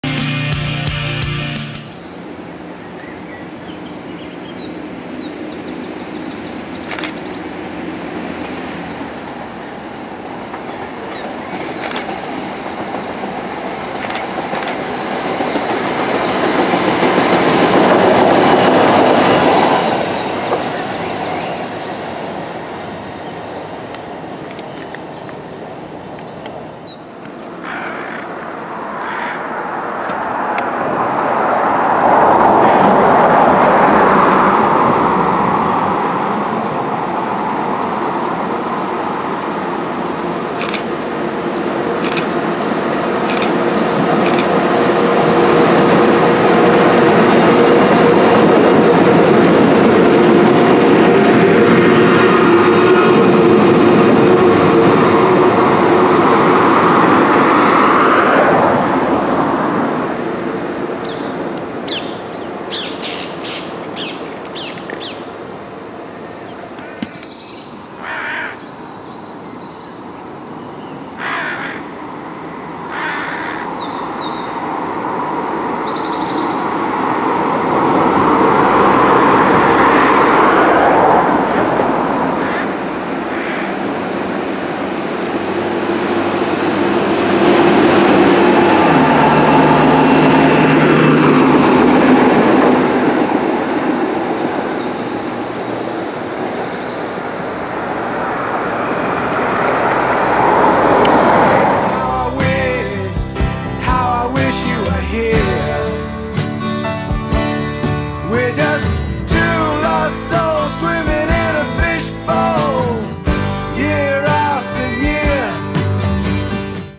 (HD) London Underground Departmental Moves at Mapesbury Road 17/06/19
In this video we see two London Underground departmental moves at Mapesbury Road. Firstly we see TMM771 pass working T771 Neasden - Ruislip Depot. Secondly we see the Schweerbau RGU Rail Grinder pass working T615 Finchley Road - Ruislip Depot. Finally we see TMM771 return working T771 Neasden - Ruislip Depot.